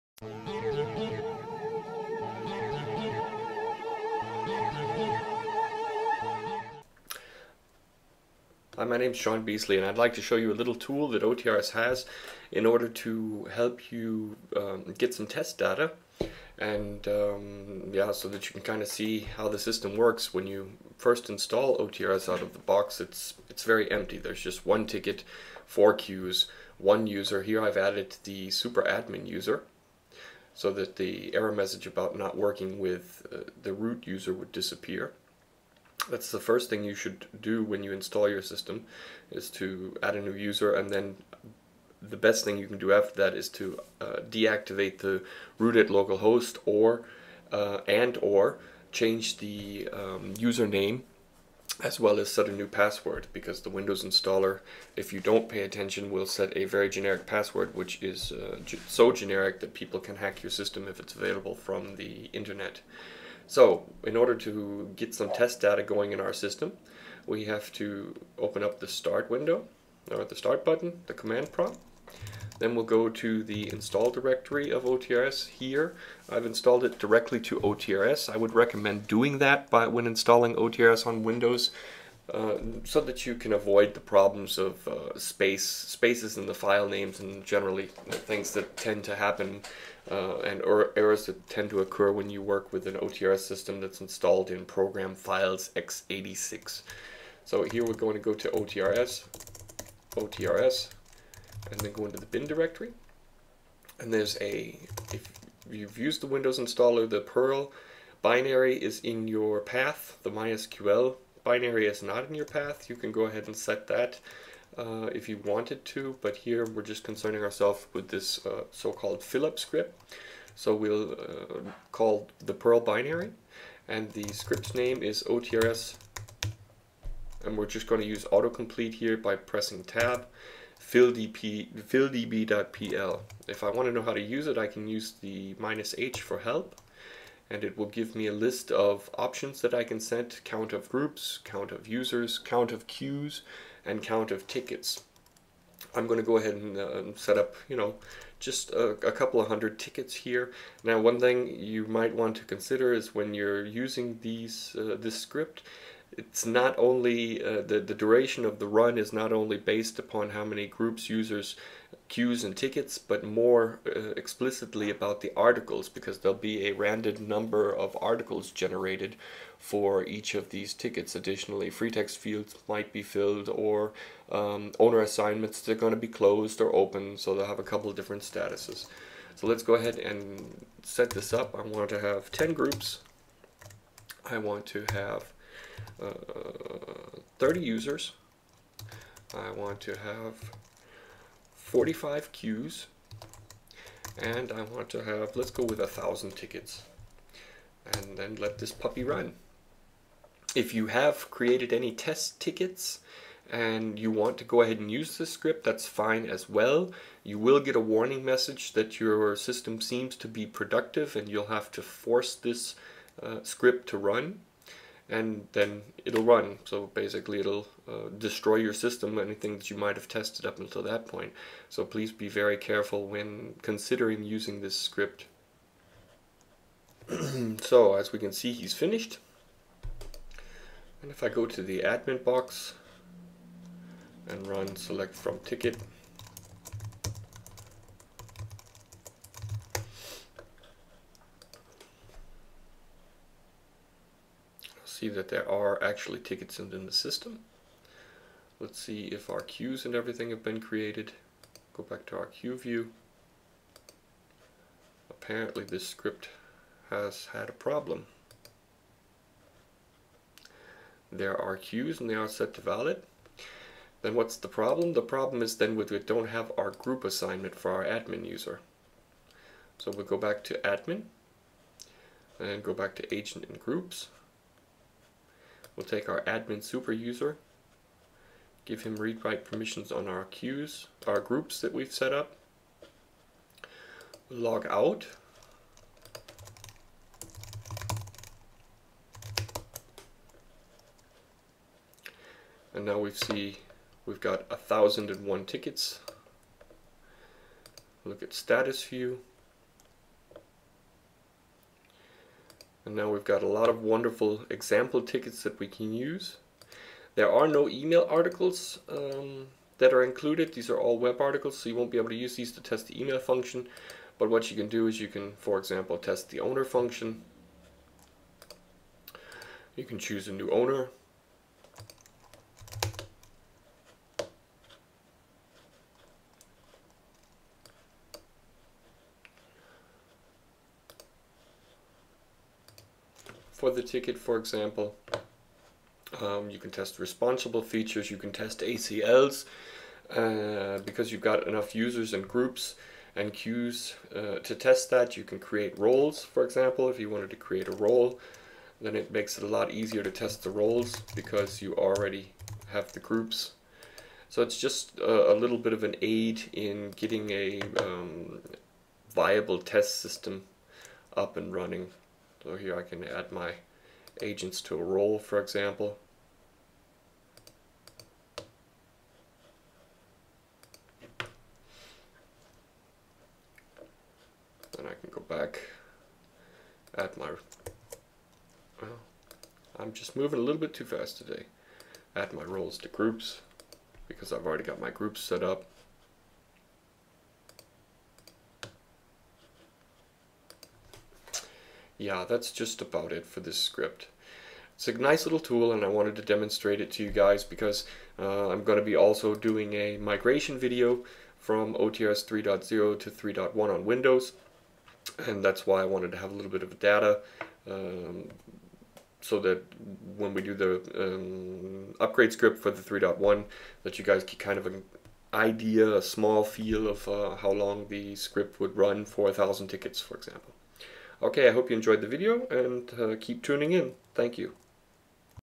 OTRS Tools: FillDB - A tool for testing.
In this video, you see a demonstration of an on-board tool for getting data into a test system, quick. This may not fit all your needs, but it does a good job creating a scenario so that you can test things like server load when searching, or how manageable a system is when you have a thousand users/groups/queues.

You can also use this to judge the time an upgrade will take.

Like this tutorial?

and sign up for our community services.